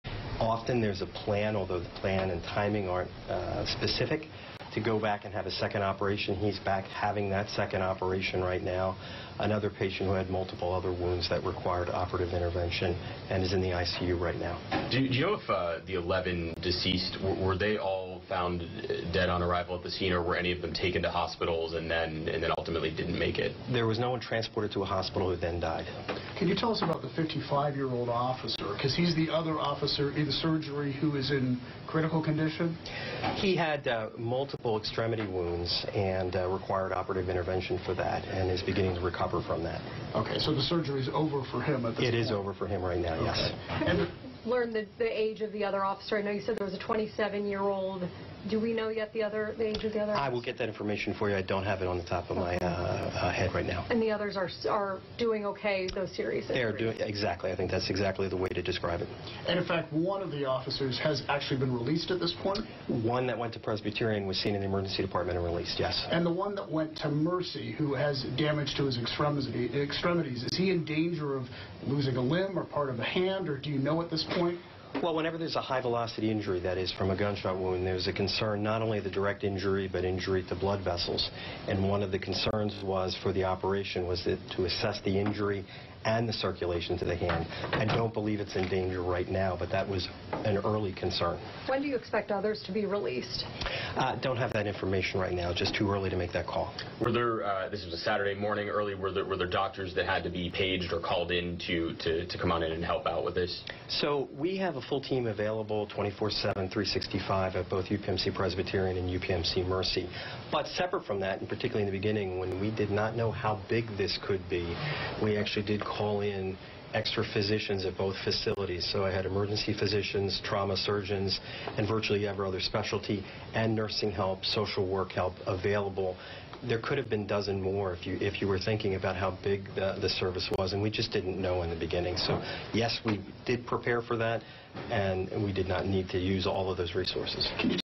Medical official says no one died after arriving to hospital, 1 officer injured has been released
Dr. Don Yealy with UPMC Presbyterian Hospital in Pittsburgh said Saturday that no one died after arriving at hospital and said one police officer injured in the Tree of Life synagogue shooting had been released.